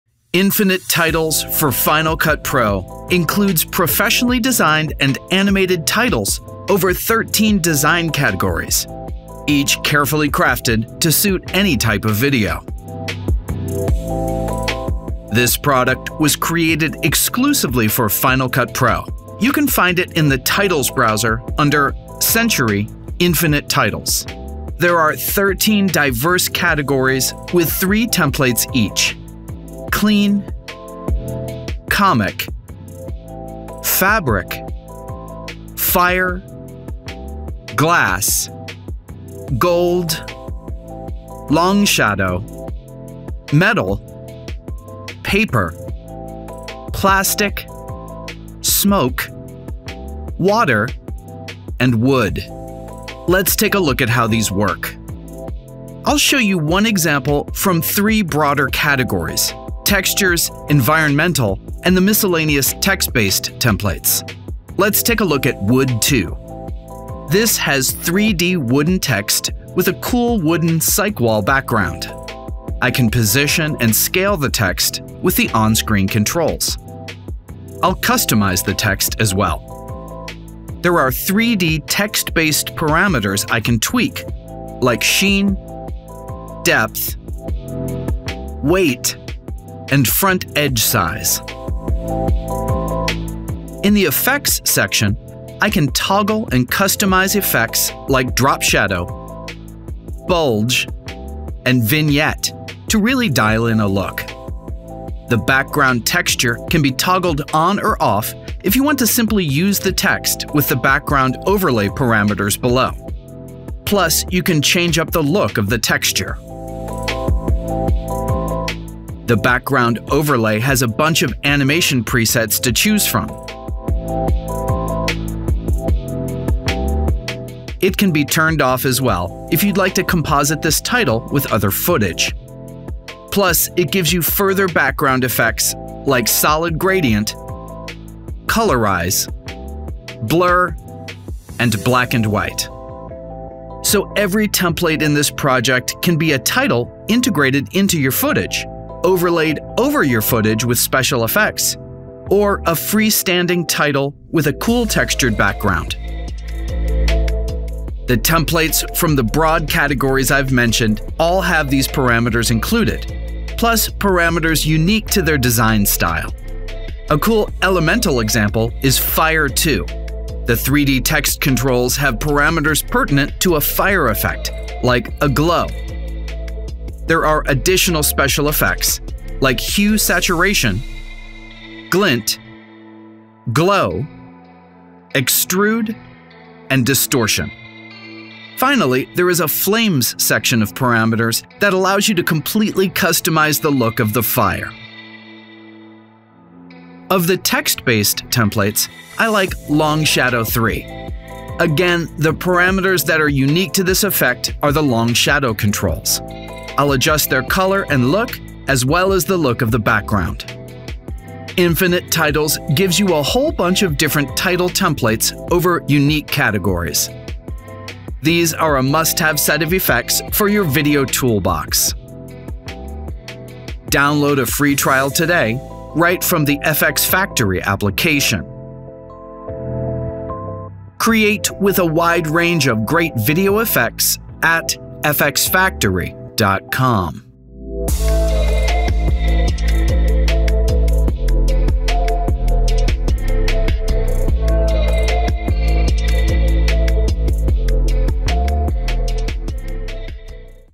First Look at Infinite Titles for Final Cut Pro
Infinite Titles for Final Cut Pro offers a complete collection of high-quality, fully customizable title templates for any video style.

With 13 distinct categories and 3 templates in each, you’ll find everything from sleek, minimalist designs to dynamic, textured titles like wood, fire, and metal.

Each template provides versatile parameters, letting you control 3D text effects, backgrounds, colors, animations, and more. Whether you’re after a bold, textured look or a subtle overlay, these titles integrate seamlessly into any project.

CHAPTERS:

00:00 Intro
00:15 Final Cut Pro Tutorial
02:37 Tutorial Example 2
03:16 Tutorial Example 3
03:31 Final Examples
03:43 How to Download
03:50 Learn More!